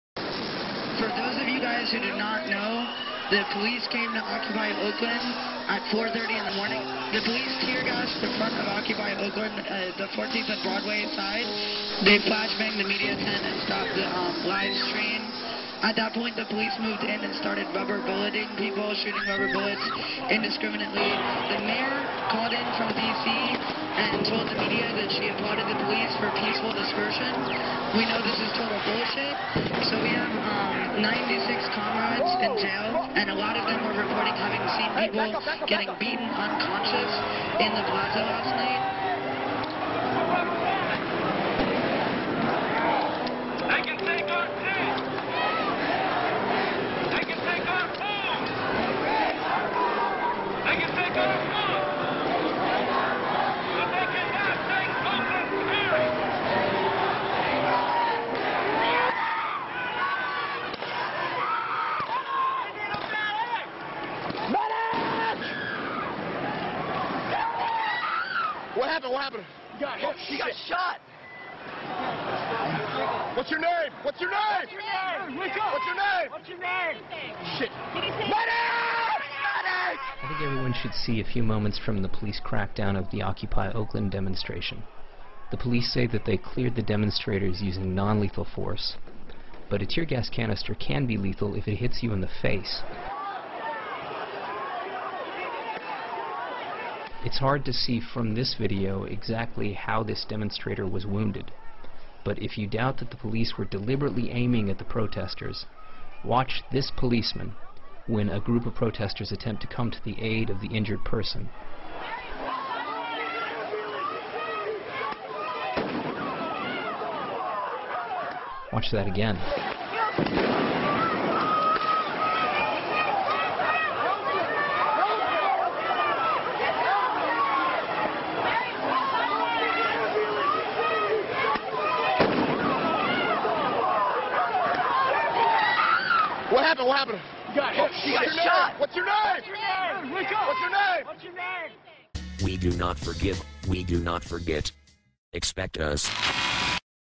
Occupy Oakland - Police violence
Tuesday, 25th of October, during a demonstration and attempt to retake the Oakland-Occupycampsite two-time Iraq war veteran, from veterans-against-the-war, got his skull fractured and risks possible permanent brain damage, allegedly during a crackdown of the police.

Teargas, rubber bullets, stungrenades and battons were used against protesters. Land of the free? Home of the brave certainly, but no the state who claims this slogan, but the people standing up while facing iminent repression and police-violence.

Google has been asked by a US law enforcement agency to remove several videos exposing police brutality from the video sharing service YouTube, the company has revealed in its latest update to an online transparency report.

Get to the streets, organize your friends, family, neightbours!

Do not forgive, do not forget. Know your enemy!